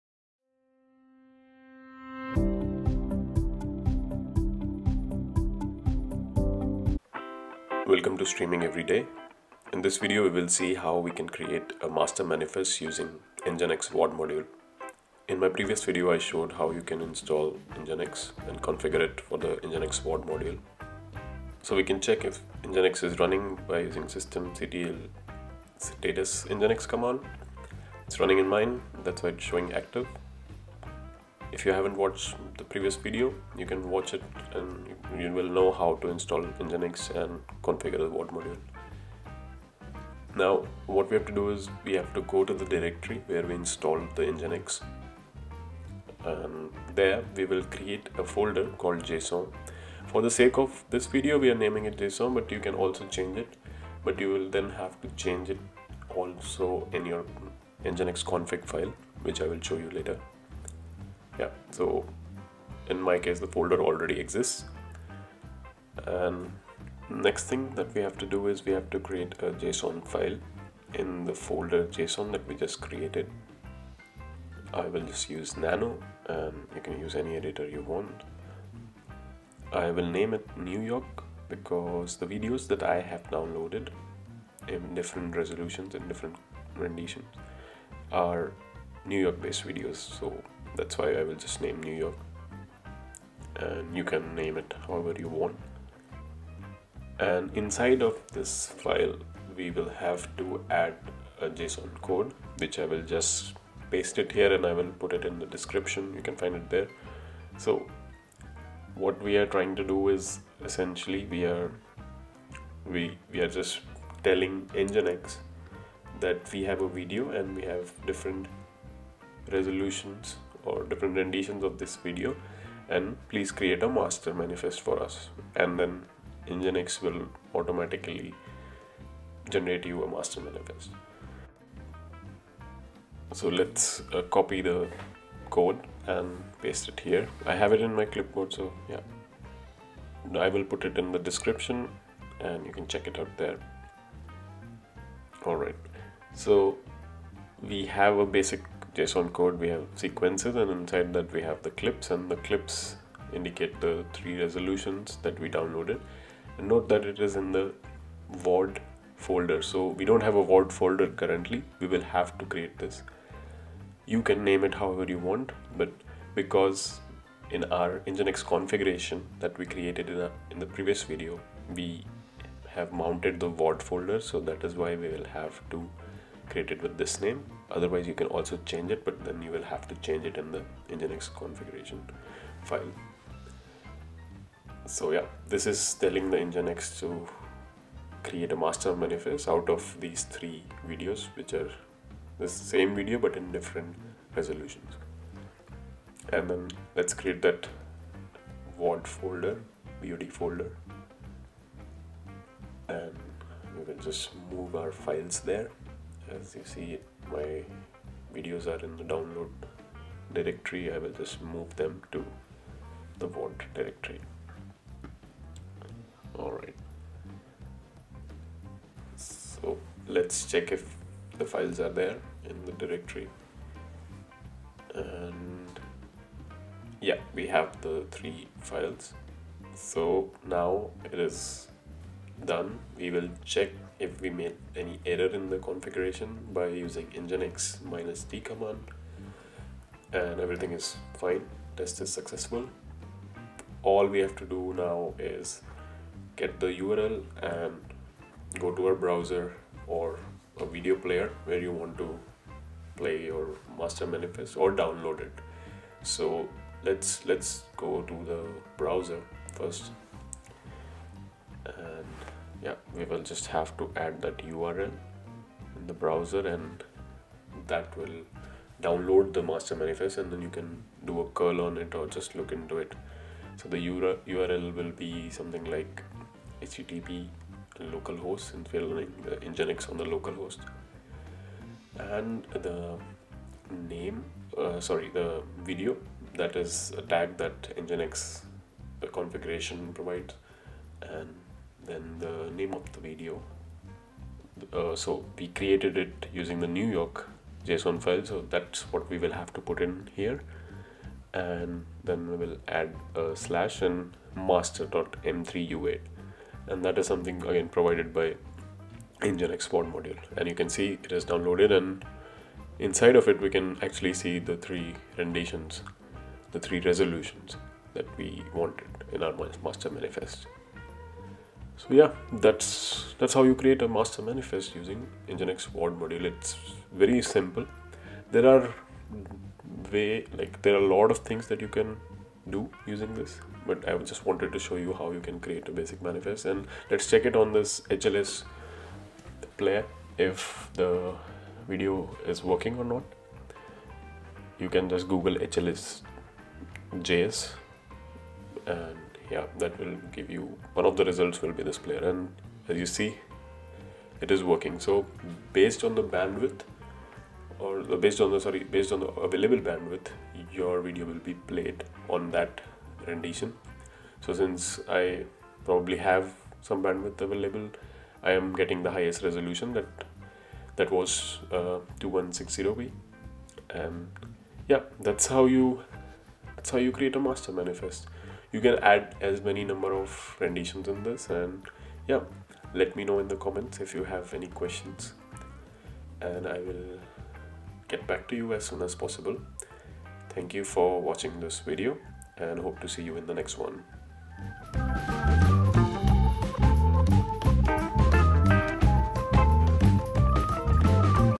Create HLS Manifest File Using Kaltura's Nginx VOD Module | Manifest Generation Using Nginx VOD
This video will guide you through the steps of creating a HLS Manifest file using Nginx VOD Module. Nginx and its VOD Module are required to installed and configured correctly for this purpose. If you help in configuring the Nginx VOD Module, then you can checkout the steps for the same in the video:

Install Kaltura's Nginx VOD Module | Install Nginx with VOD Module on Ubuntu 20.04 | Easy Tutorial